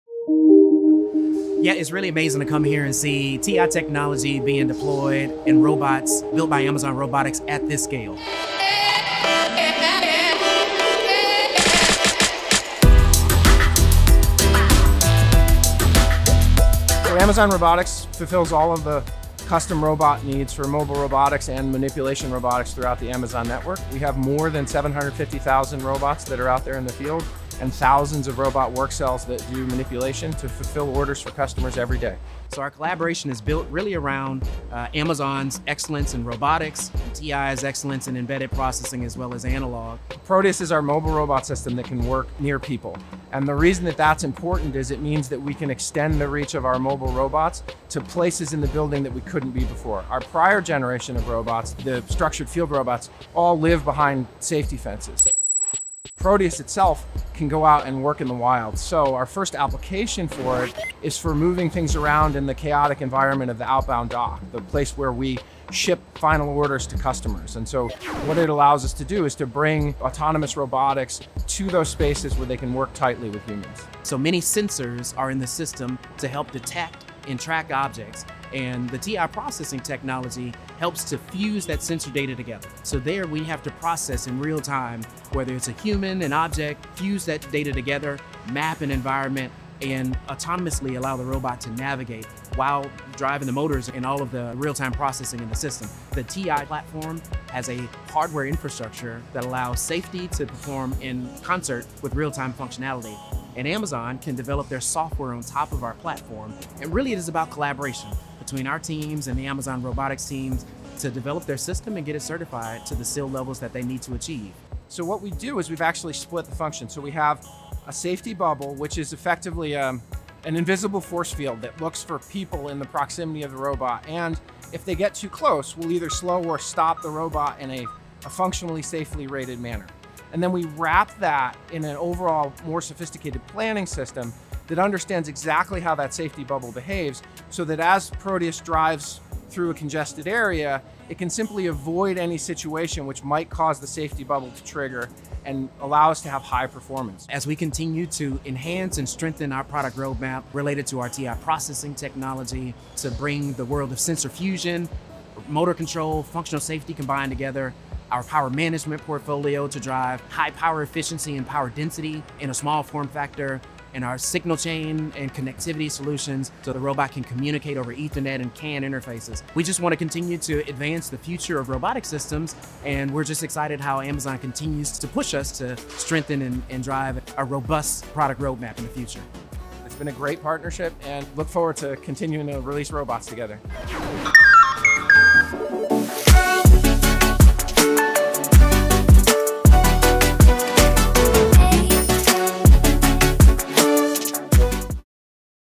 Amazon 與 TI 攜手打造更安全、更智慧的機器人
Amazon 是最大的機器人製造商之一，在營運中使用了超過 750,000 個機器人。從 Hercules 到全自動的 Proteus，Amazon 和 TI 共同開發了可在 Amazon 倉庫中發揮功用的多種機器人，這些機器人都使用了 TI 著重於功能安全和感測器融合的處理、連線和電源技術。使用 TI 專為感知感測、精準馬達控制、即時通訊和 AI 技術設計的技術，讓您的機器人設計成真。
利用 TI 技術，讓您的機器人設計成真